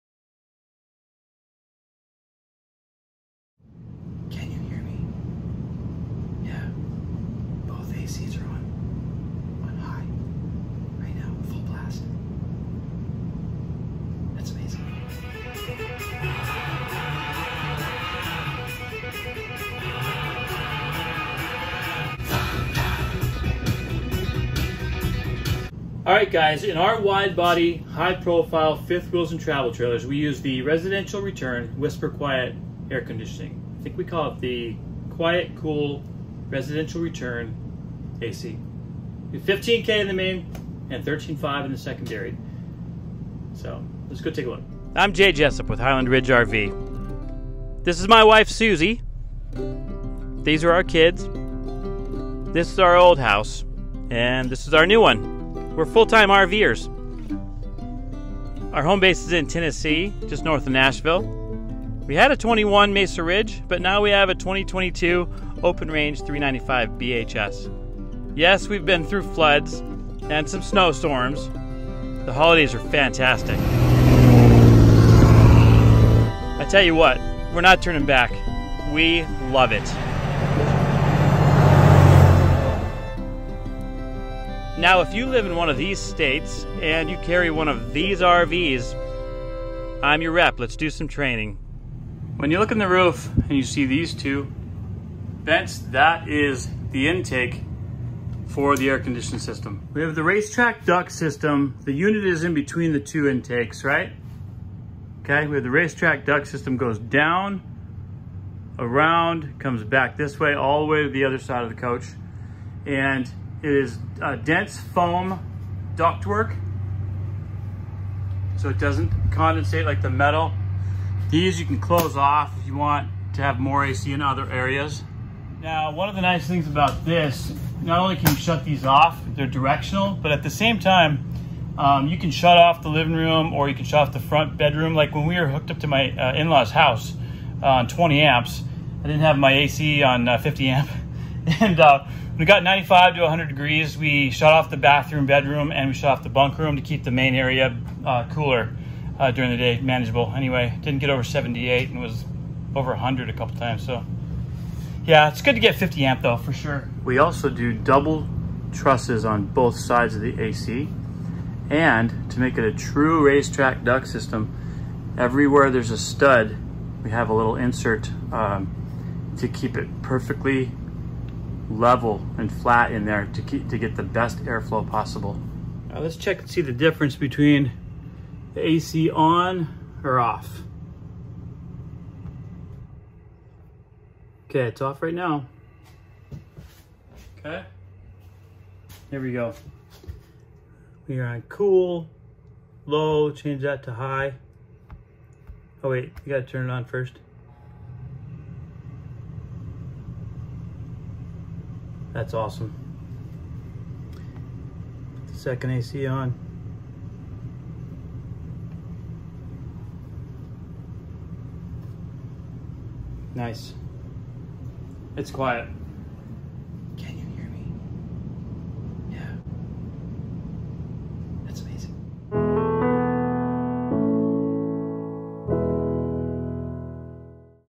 Highland Ridge RV RESIDENTIAL RETURN WHISPER QUIET AC
All of our Highland Ridge RV Open Range and Mesa Ridge full profile fifth wheels and travel trailers have the Residential Return Quiet Cool AC which actually is “Whisper Quiet”. It’s quieter than whispering because you can whisper a conversation when both ACs are on full blast!! Check it out!